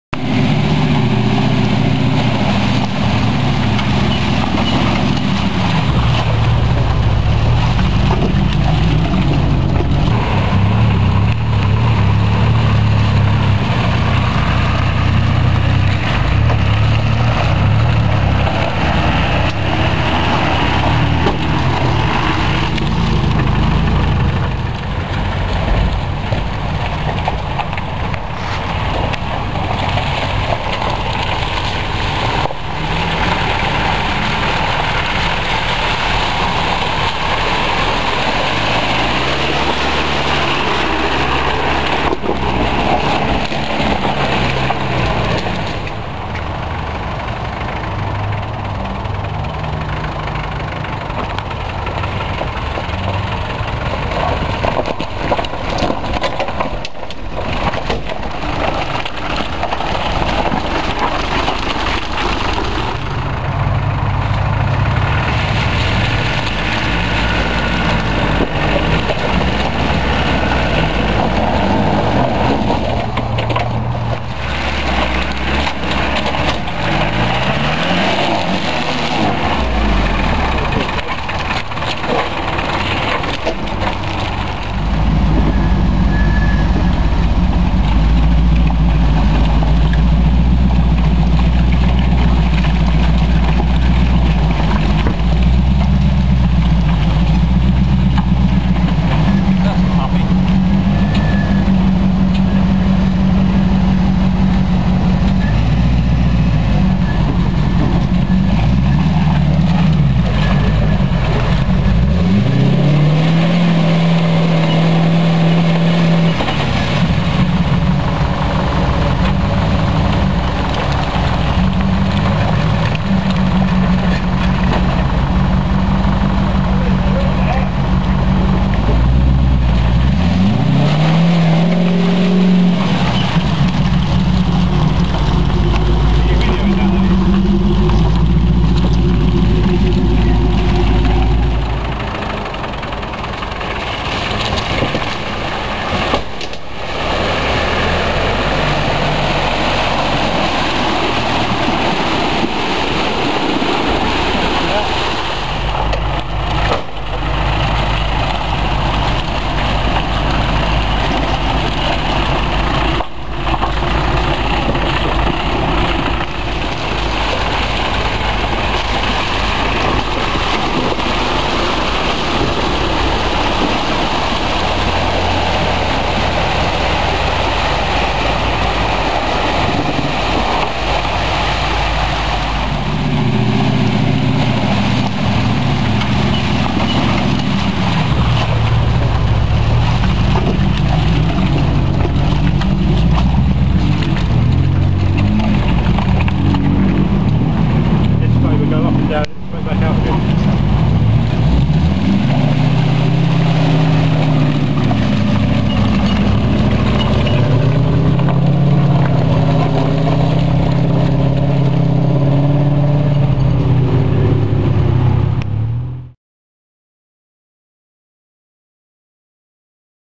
SLRC Brick Kiln Farm November 2007
Re-edit of footage already existing as separate videos. I've no idea why this has gone widescreen as the source footage was 4:3 and it was processed as 4:3 with direct upload from PowerDirector...

All of the footage is from my old Canon Powershot, so there is no zoom.